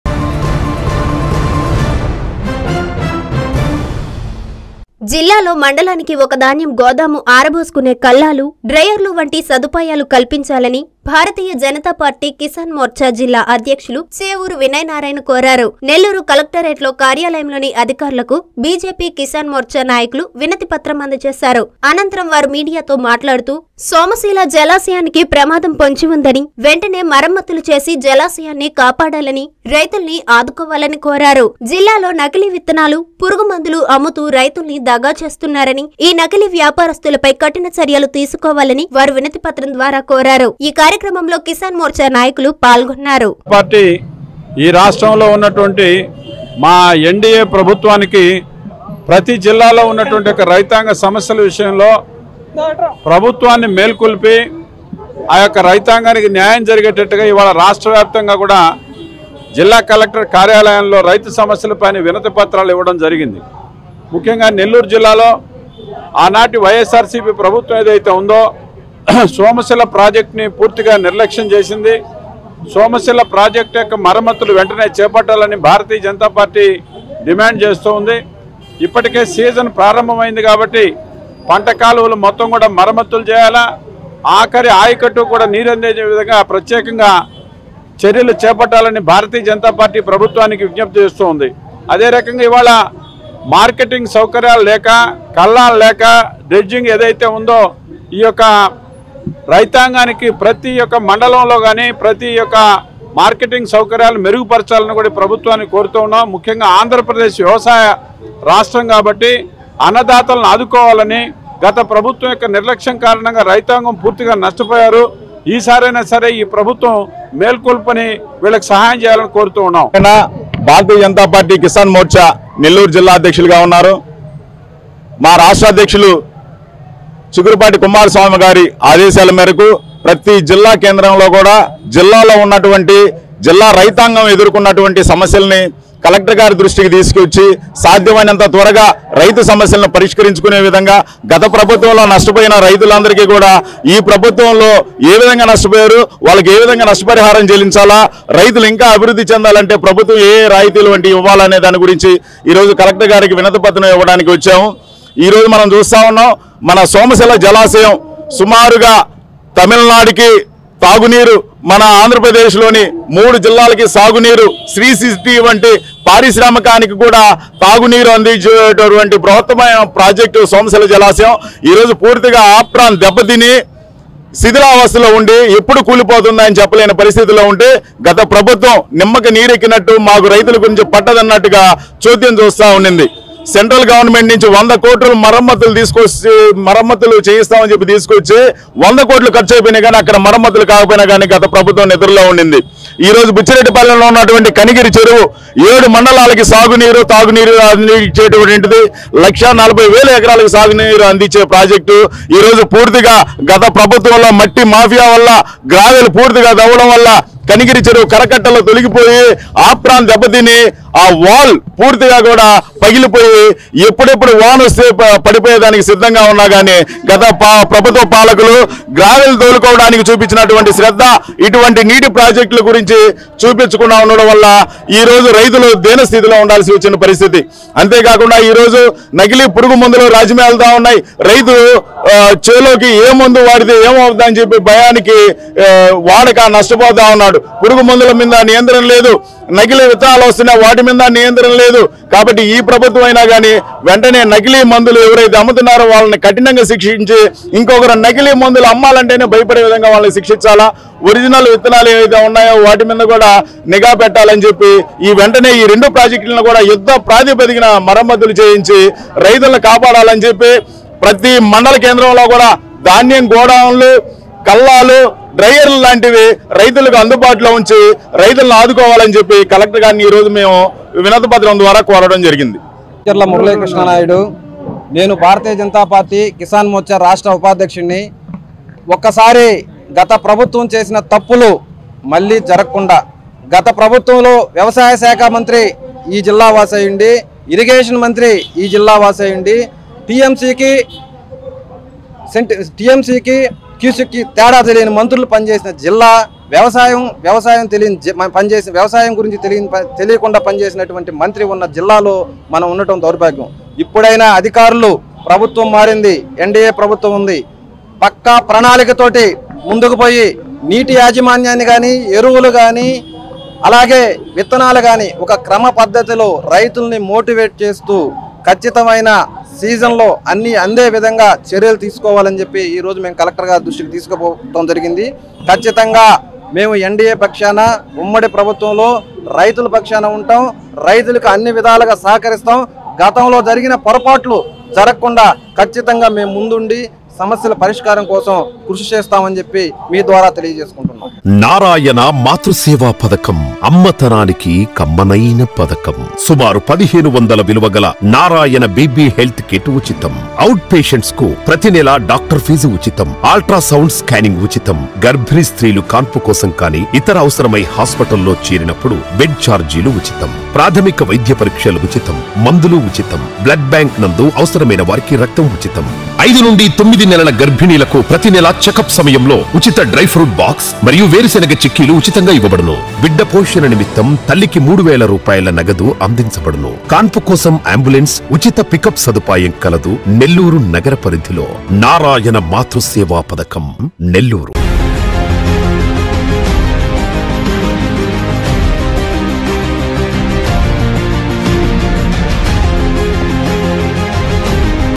వ్య‌వ‌సాయ రంగానికి మౌలిక వ‌స‌తులు క‌ల్పించండి | BJP Kisan Morcha | Collector's Office | Nellore
- ప్ర‌మాద‌పుటంచున ఉన్న సోమ‌శిల జ‌లాశ‌యానికి మ‌ర‌మ్మ‌తులు చేయించండి
- క‌లెక్ట‌ర్ కార్యాల‌యంలో విన‌తి ప‌త్రం అంద‌చేసిన బీజేపీ కిసాన్ మోర్చా